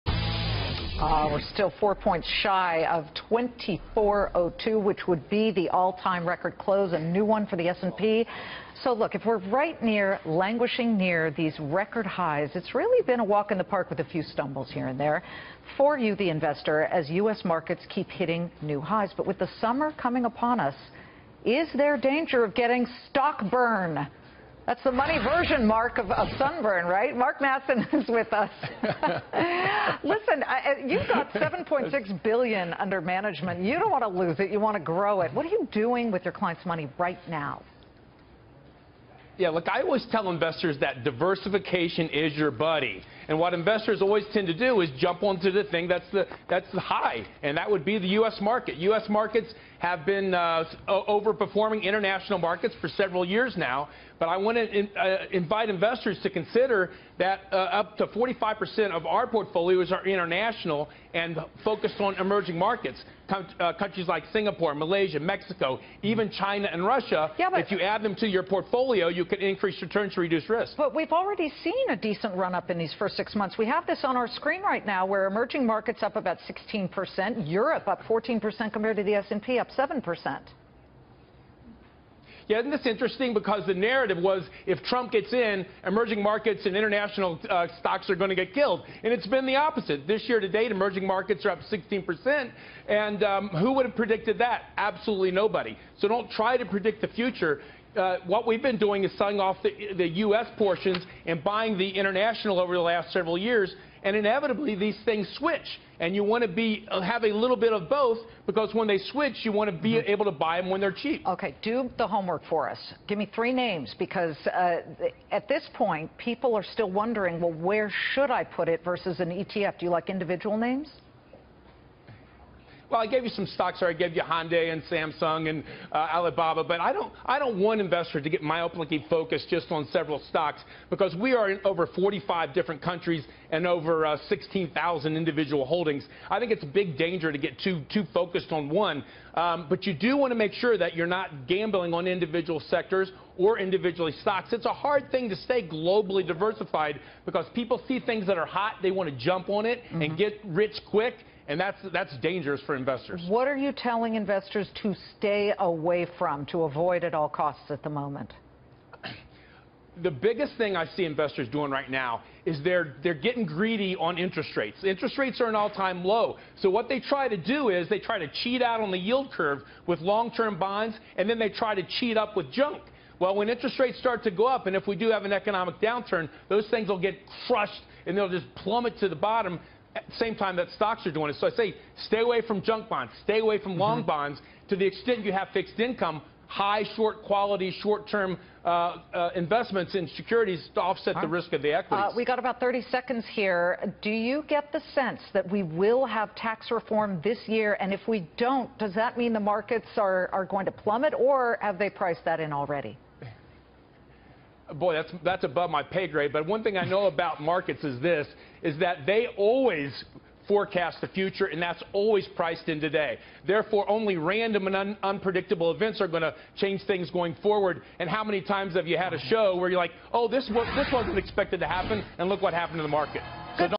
Is it time to invest overseas?
Matson Money CEO Mark Matson on whether it’s time to invest in overseas markets.